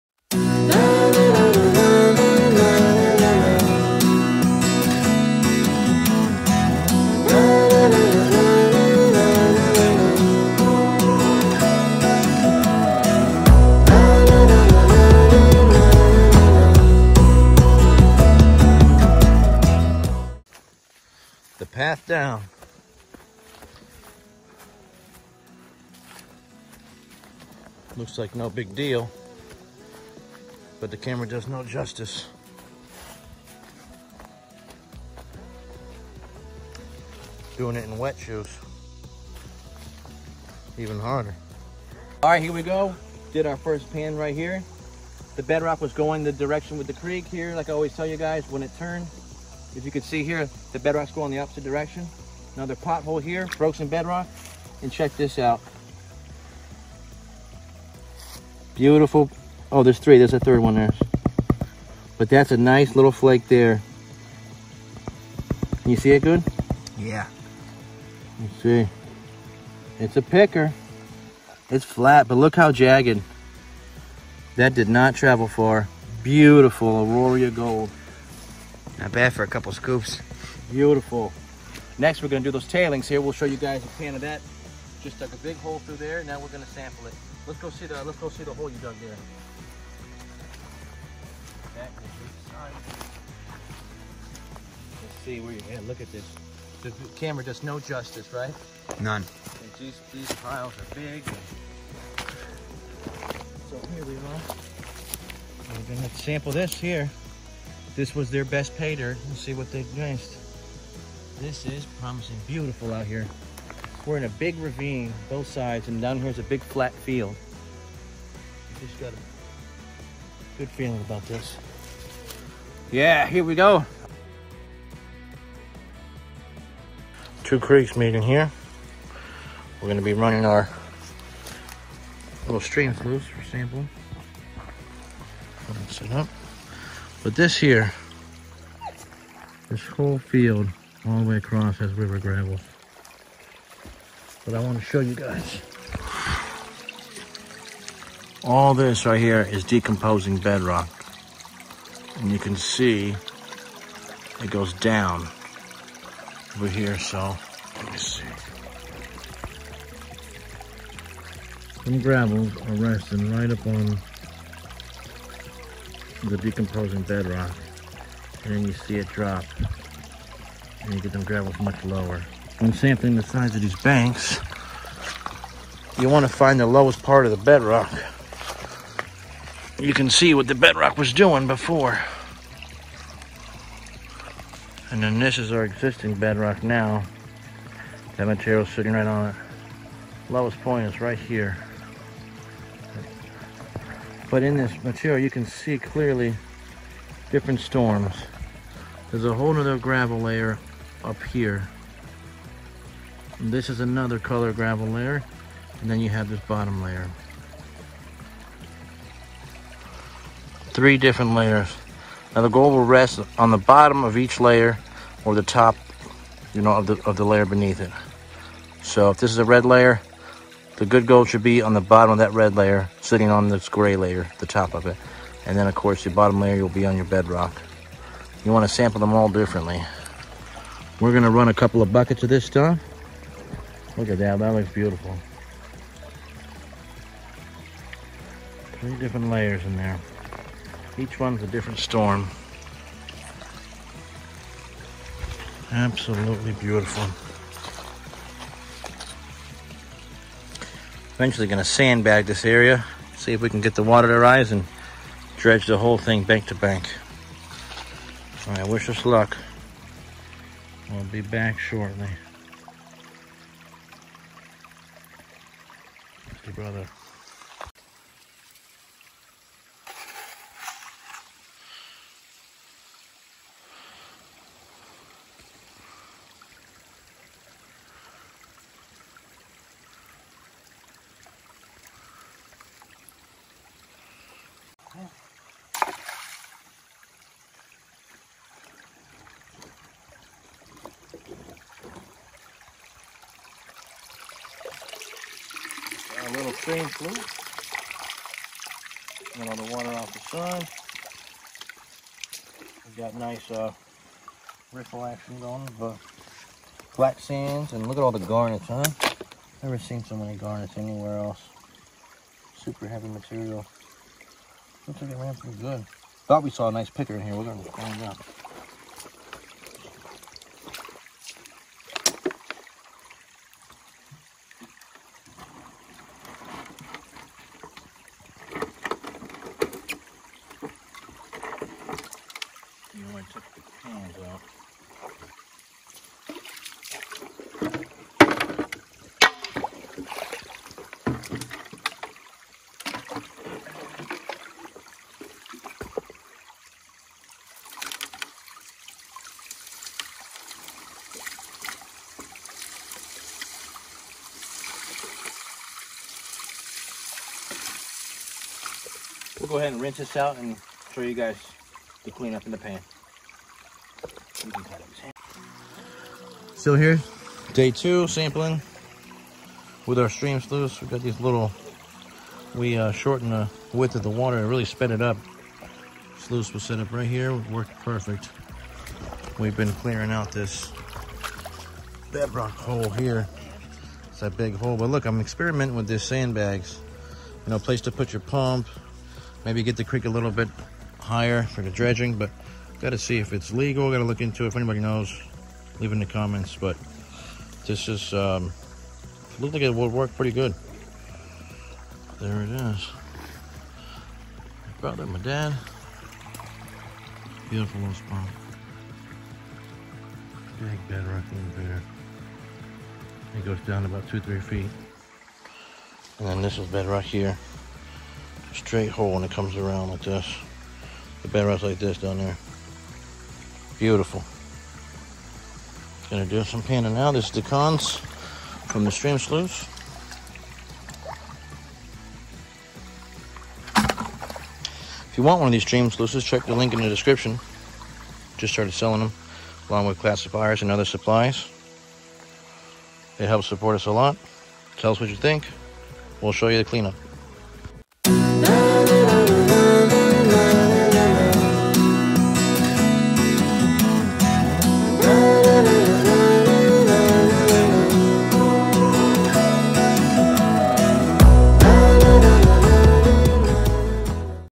How to Read & Prospect Old River Channels
Here we are just doing some more sampling around this old gold mine!
We are back full force on YouTube and will be building this channel up!

Prospecting gear used in the video.